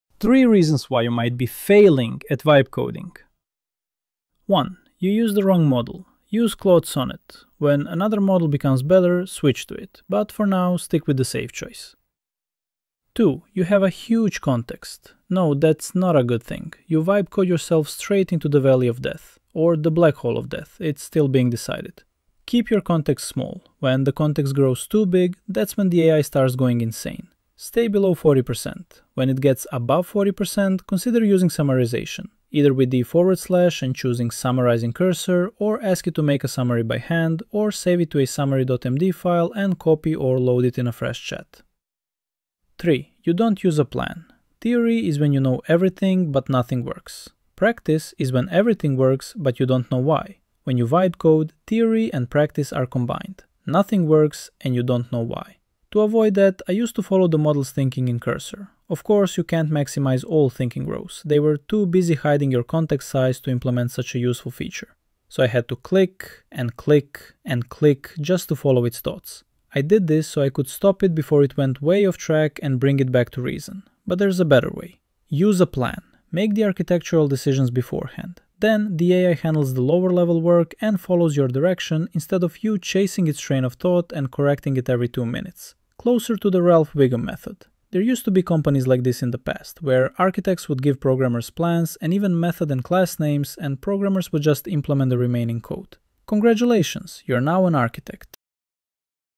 3 reasons why you might be failing at vibe coding
Why you might be failing at vibe coding:

1. Wrong model
2. Huge conetext
3. Not using a plan

With cursor screens.